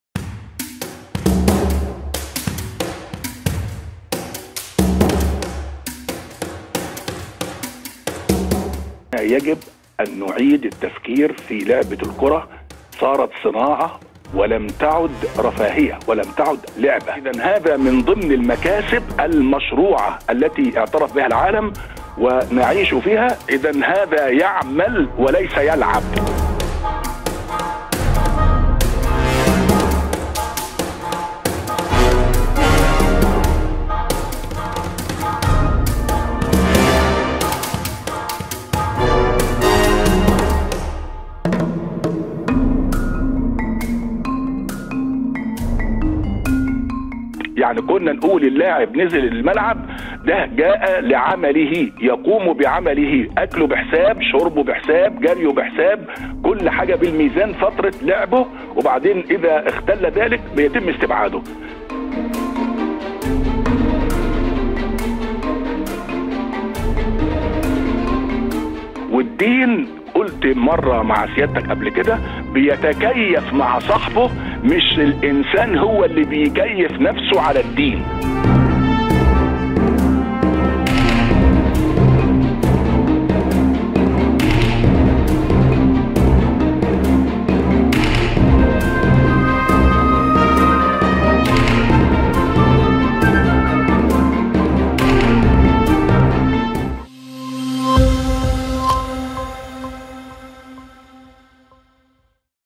سعد الهلالي: إفطار اللاعب في نهار رمضان جائز وفق تعليمات المدرب
مونتاج: عمرو فرج